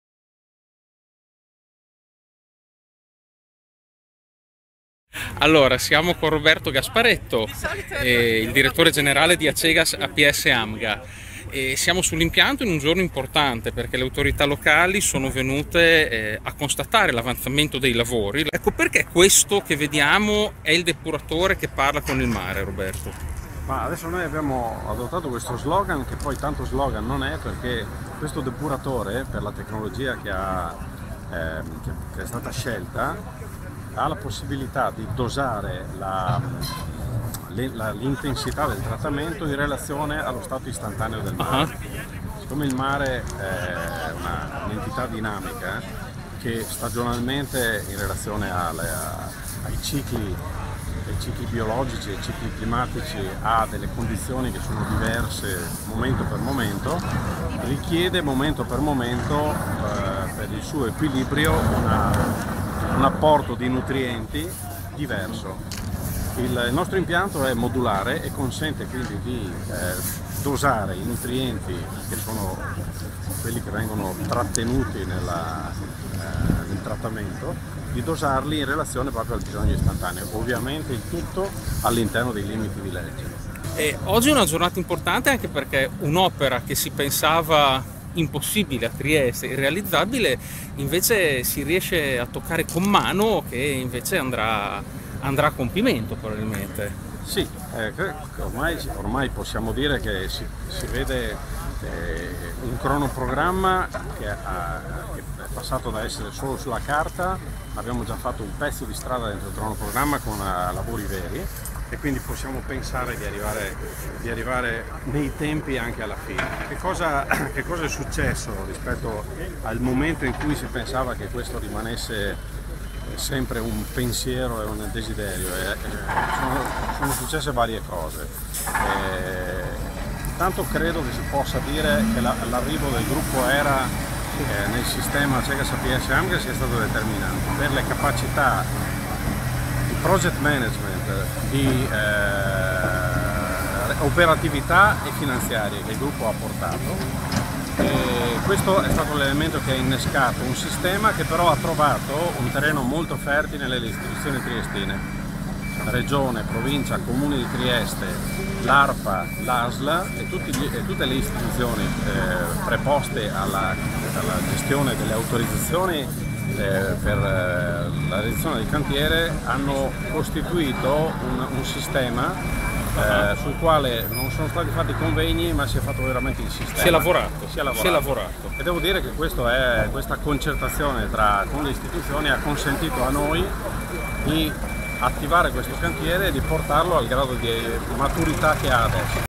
20160411 VisitaServola Gasparetto
Cantiere AcegasApsAmga del nuovo depuratore di Servola - Trieste. Visita delle autorità locali per la verifica dello stato di avanzamento lavori dell'11 aprile 2016.  Roberto Gasparetto, Direttore Generale AcegasApsAmga, illustra lo stato di avanzamento lavori